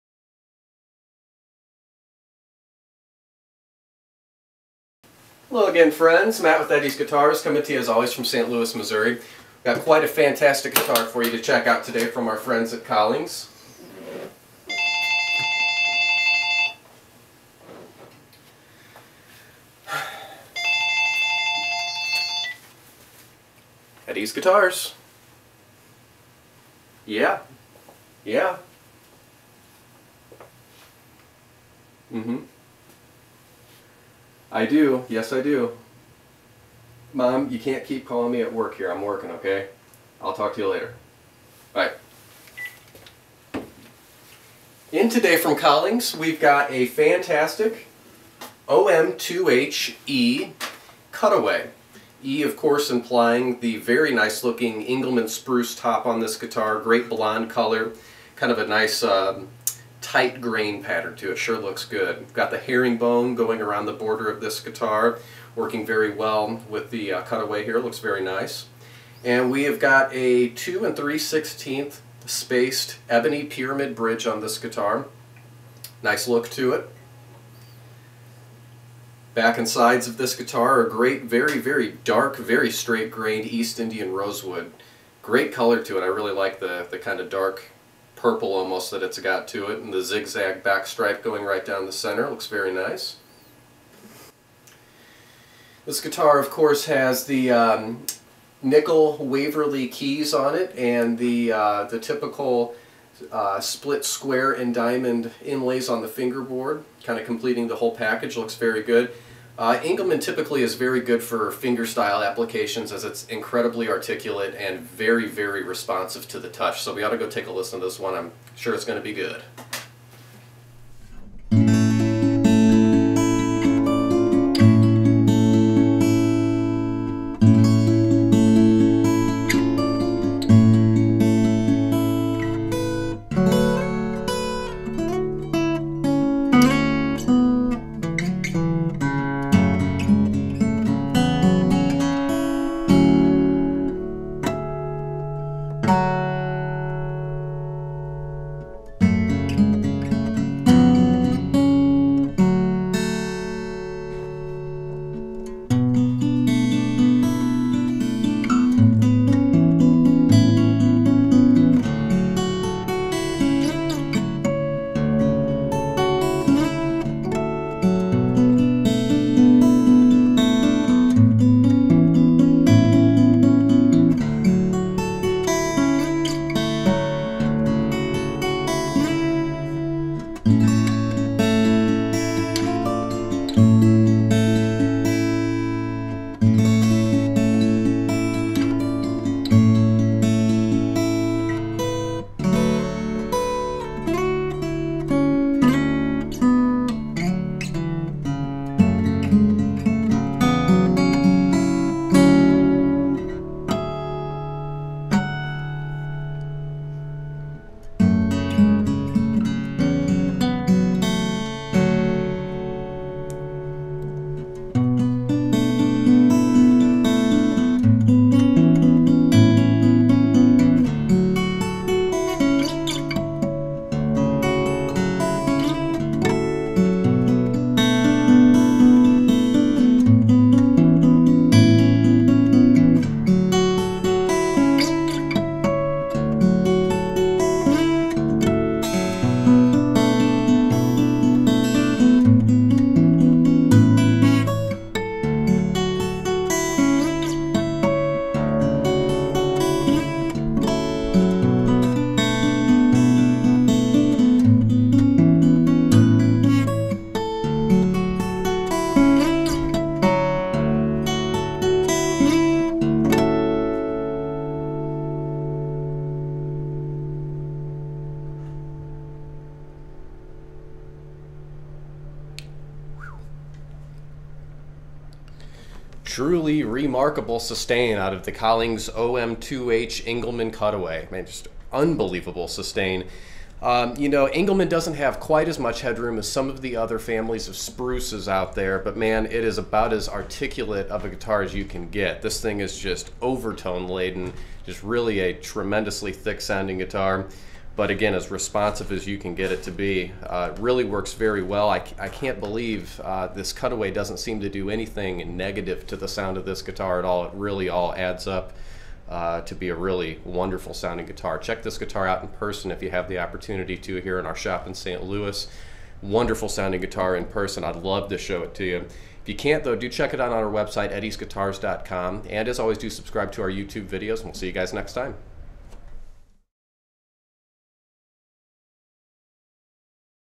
Collings OM2HE Cutaway
New Acoustic Guitar Demo at Eddie's Guitars in St. Louis, MO.
Here is a spectacular OM that any fingerstylist will certainly go nuts over. Engelmann top, pyramid bridge and a cutaway.. all excellent ingredients for a delicate, yet complex sounding guitar. This one in particular has some of the most impressive sustaining overtones I've heard in this sized guitars. The sound seems to bloom and almost gain momentum the longer it rings out... very rare in a brand new instrument!!